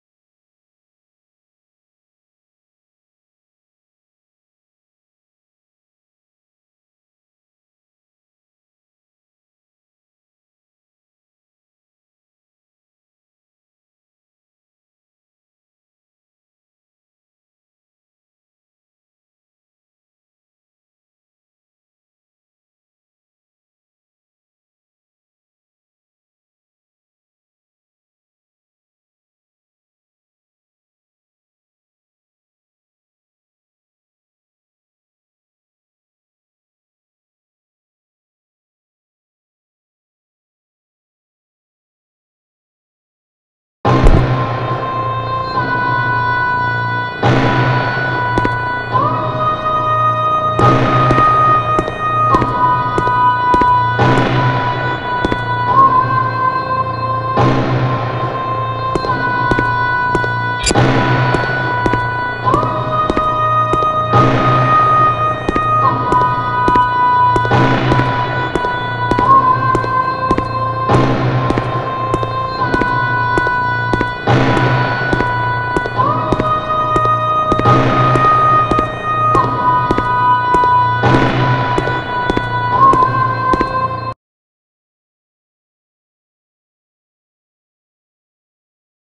Scariest mobile app of 2016
This is a pretty scary app that I made, I'll be scaring a horror series on how to make a scary app like this or making a game like slender man.